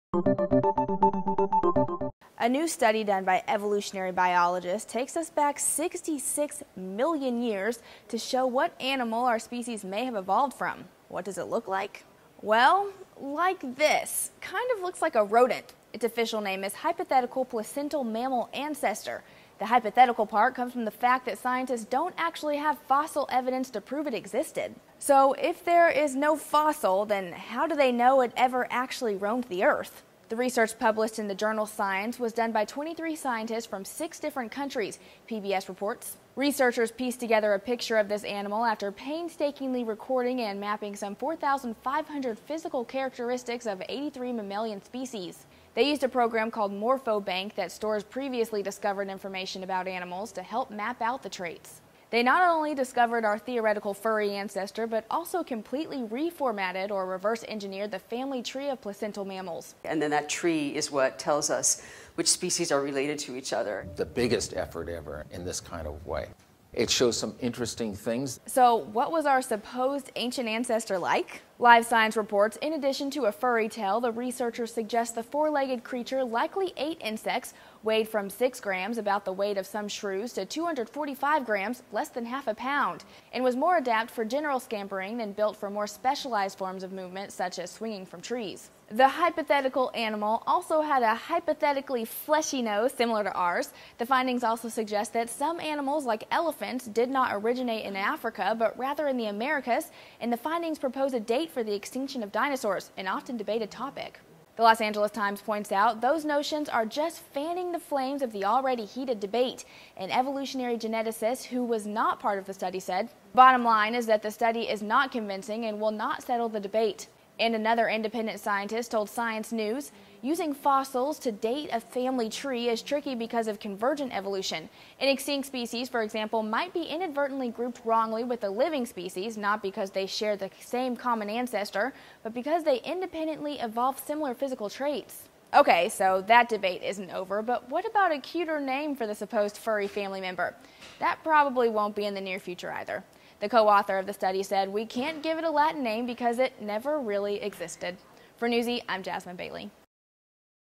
Scientist Generate 'Hypothetical' Ancestor To Mammals
Evolutionary biologist around the world used fossil information to piece together the 'Hypothetical Placental Mammal Ancestor.'